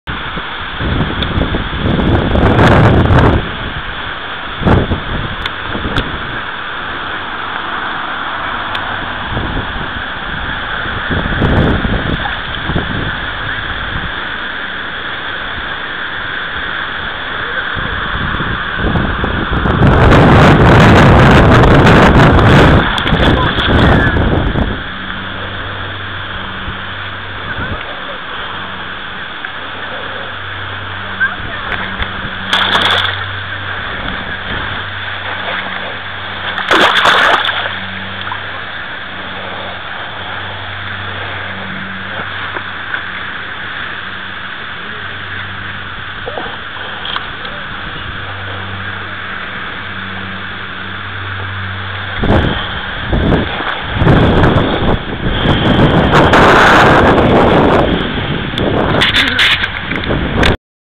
snoek 63 cm deel 2
dit is deel 2 van de snoek van 63 cm. zo dom dat ik cam uit heb gezet. maar toch flink deel vast weten te leggen.